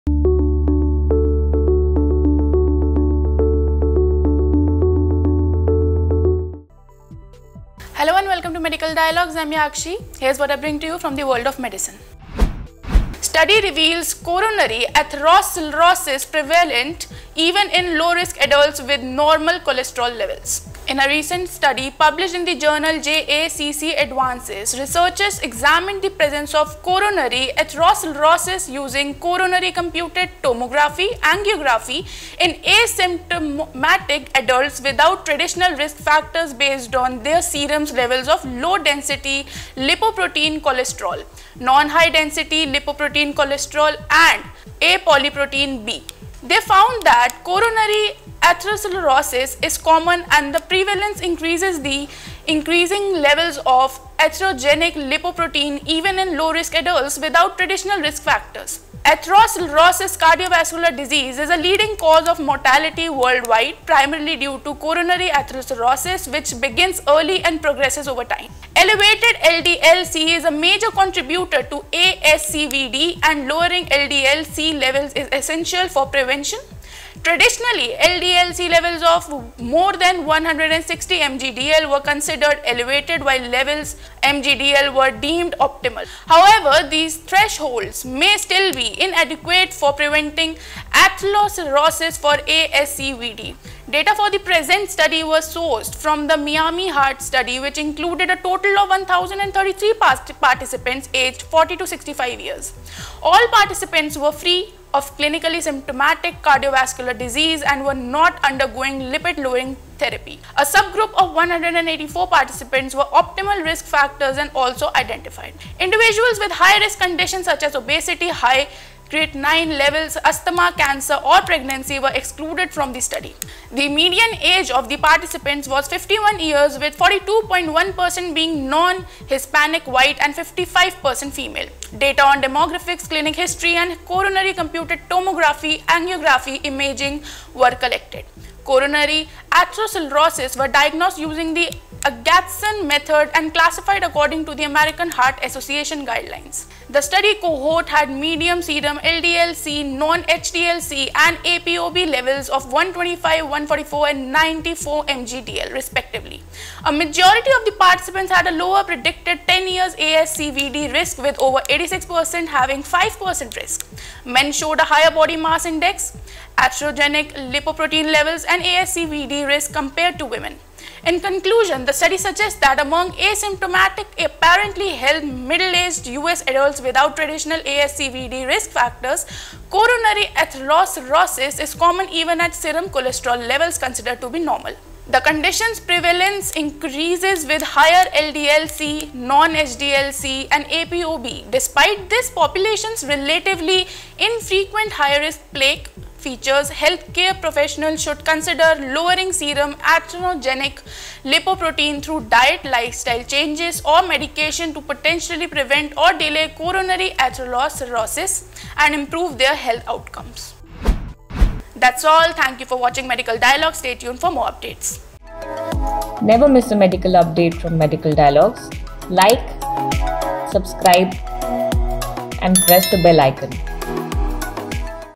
reveals coronary atherosclerosis prevalent even in low-risk adults with normal cholesterol levels
In a recent study published in the journal JACC Advances, researchers examined the presence of coronary atherosclerosis using coronary computed tomography angiography (CCTA) in asymptomatic adults without traditional risk factors based on their serum levels of low-density lipoprotein cholesterol (LDL-C), non-high-density lipoprotein cholesterol (non-HDL-C), and apolipoprotein B (apoB). They found that coronary atherosclerosis is common, and the prevalence increases with increasing levels of atherogenic lipoproteins, even in low-risk adults without traditional risk factors.
Atherosclerotic cardiovascular disease (ASCVD) is a leading cause of mortality worldwide, primarily due to coronary atherosclerosis, which begins early and progresses over time. Elevated LDL-C is a major contributor to ASCVD, and lowering LDL-C levels is essential for prevention. Traditionally, LDL-C levels of 160 mg/dL were considered elevated, while levels 100 mg/dL were deemed optimal. However, these thresholds may still be inadequate for preventing atherosclerosis or ASCVD.
Data for the present study were sourced from the Miami Heart (MiHeart) study, which included a total of 1,033 participants aged 40–65 years. All participants were free of clinically symptomatic cardiovascular disease and were not undergoing lipid-lowering therapy. A subgroup of 184 participants with optimal risk factors was also identified. Individuals with high-risk conditions such as obesity, high creatinine levels, asthma, cancer, or pregnancy were excluded from the study. The median age of the participants was 51 years, with 42.1% being non-Hispanic White, and 55% female. Data on demographics, clinical history, and coronary computed tomography angiography (CCTA) imaging were collected. Coronary atherosclerosis was diagnosed using the Agatson method and classified according to the American Heart Association guidelines.
The study cohort had median serum LDL-C, non-HDL-C, and apoB levels of 125, 144, and 94 mg/dL, respectively. A majority of the participants had a lower predicted 10-year ASCVD risk, with over 86% having 5% risk. Men showed a higher body mass index, atherogenic lipoprotein levels, and ASCVD risk compared to women.
In conclusion, the study suggests that among asymptomatic, apparently healthy, middle-aged US adults without traditional ASCVD risk factors, coronary atherosclerosis is common even at serum cholesterol levels considered to be normal. The condition's prevalence increases with higher LDL-C, non-HDL-C, and apoB. Despite this population's relatively infrequent high-risk plaque features, healthcare professionals should consider lowering serum atherogenic lipoproteins through diet, lifestyle changes, or medication to potentially prevent or delay coronary atherosclerosis and improve their health outcomes.